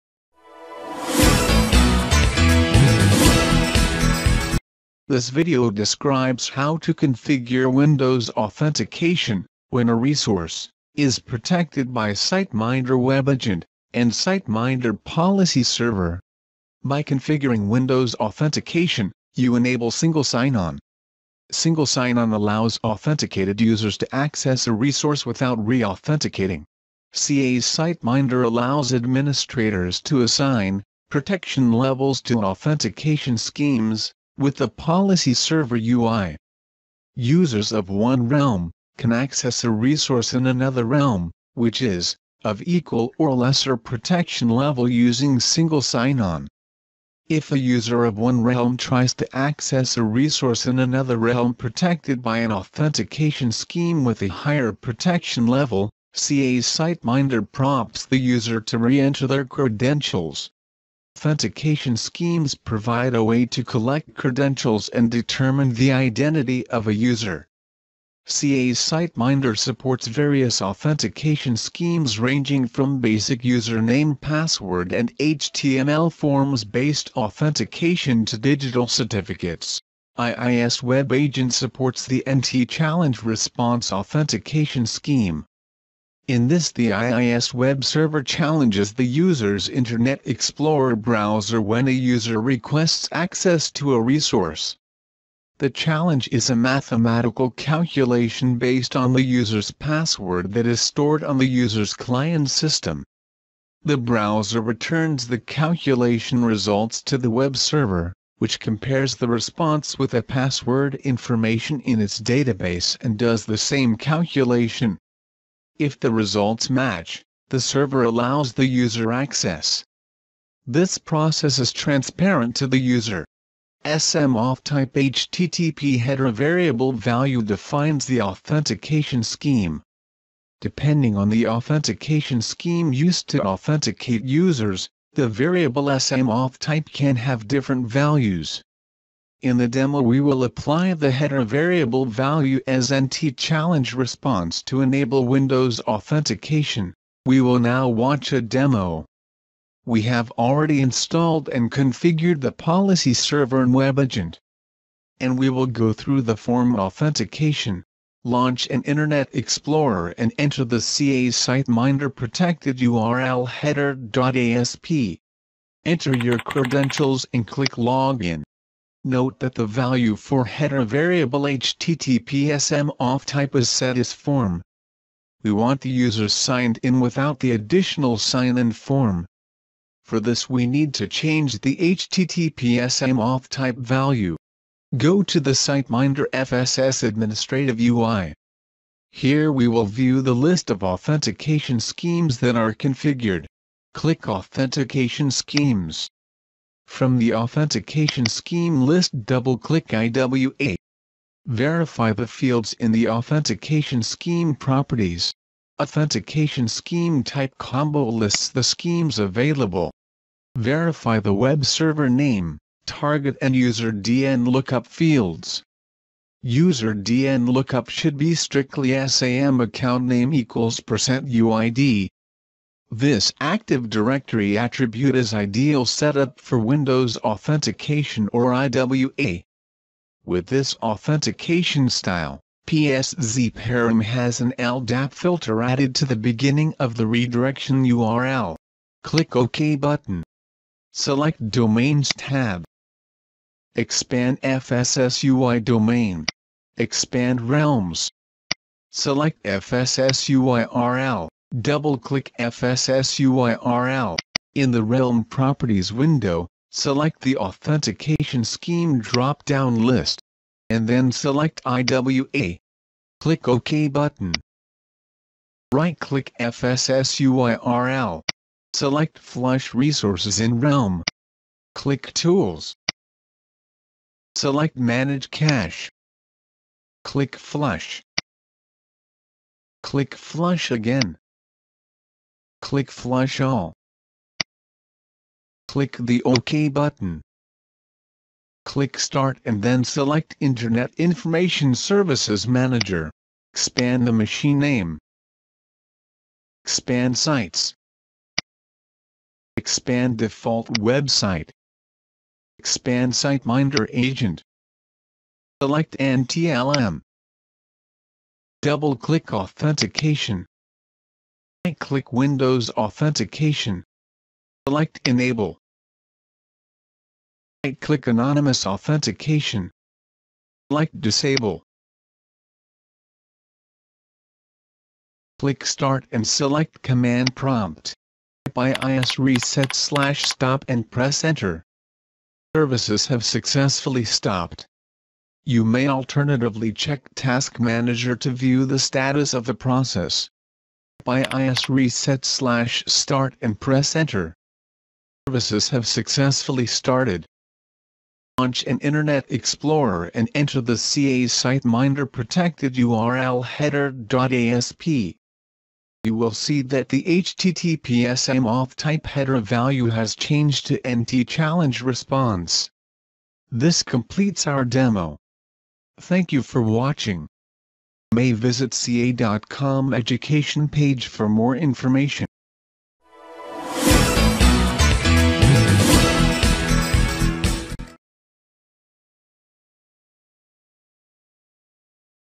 CA Single Sign-On (formerly known as CA SiteMinder®) - How to Configure Windows Authentication
How to configure Windows authentication in CA Single Sign-On.